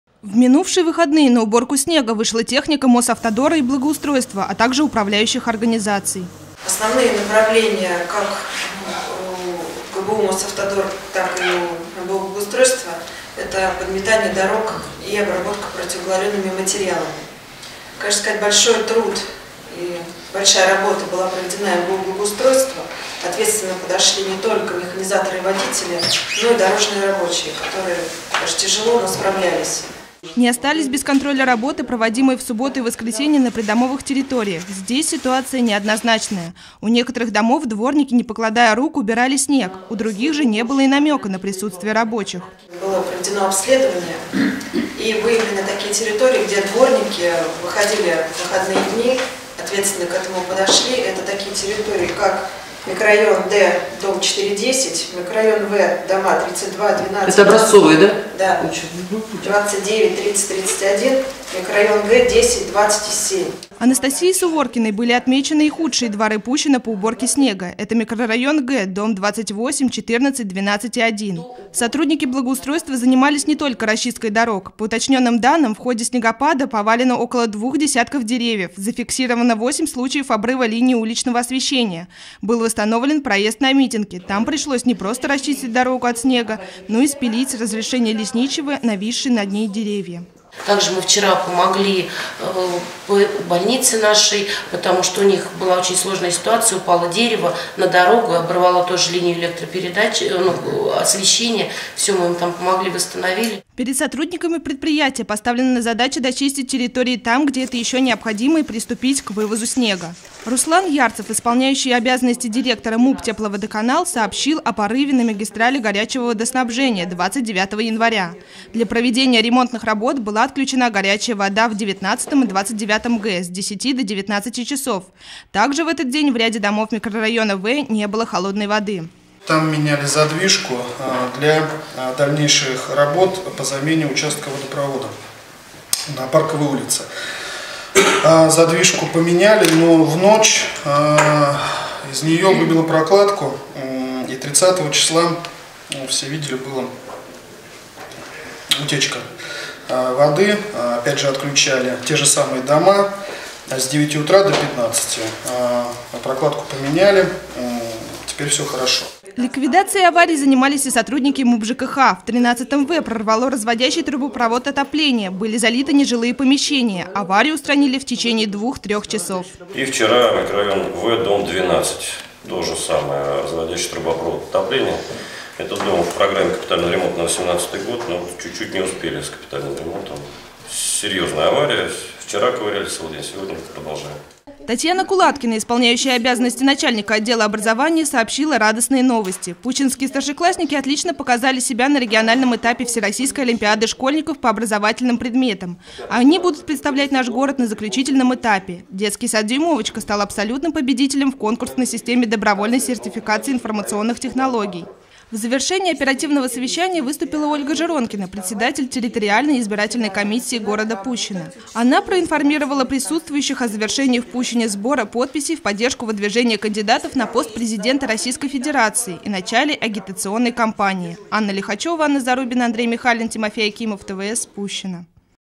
Вести с планерки 6 февраля
Сильнейший снегопад позади. О том, как боролись с его последствиями, какие еще чрезвычайные ситуации произошли на истекшей неделе, докладывали руководители различных служб на оперативном совещании 6 февраля в администрации Пущина.